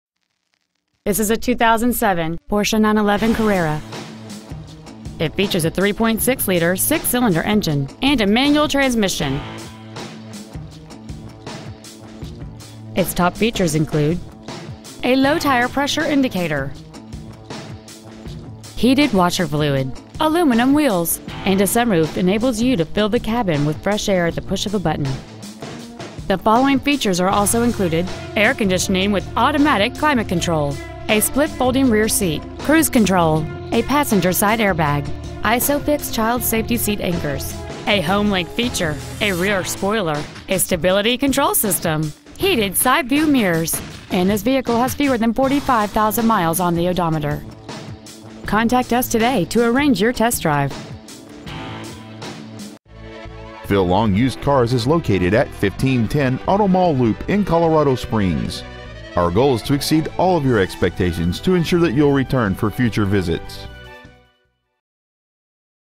2007 Porsche 911 Colorado Springs CO
We are proud to present this 2007 Porsche 911 .

 Year : 2007
 Make : Porsche
 Model : 911
 Engine : 3.6L HO DOHC 24v 6-Cyl Engine
 Trans . : Manual

 Miles : 44,880

 Pre-Owned 2007 Porsche 911 Colorado Springs CO